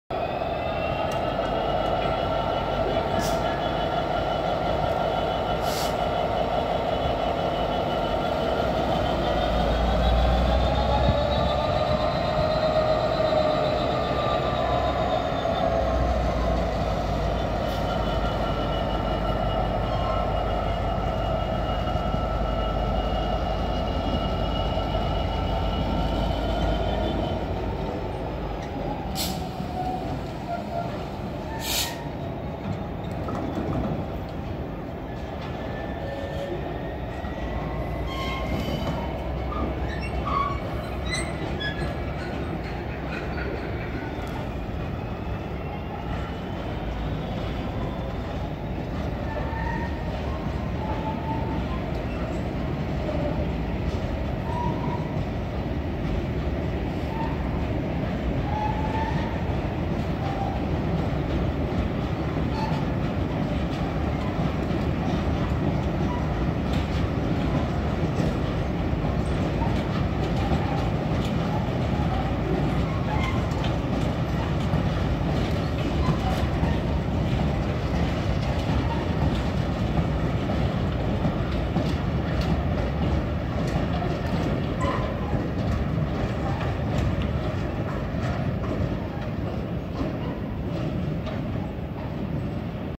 Shed at Sheffield
Freightliner Class 66 number 66610 departs Sheffield platform 8 on the 6G87 Dewsbury Blue Circle to Earles sidings.
The train was comprised of PCA wagons.